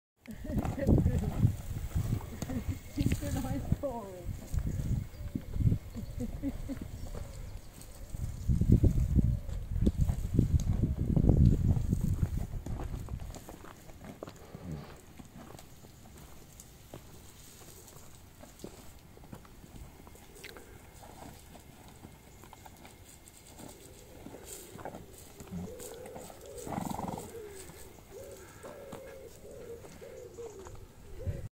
Tom and Henry having a shower
Mainly Henry having a shower on a very hot day.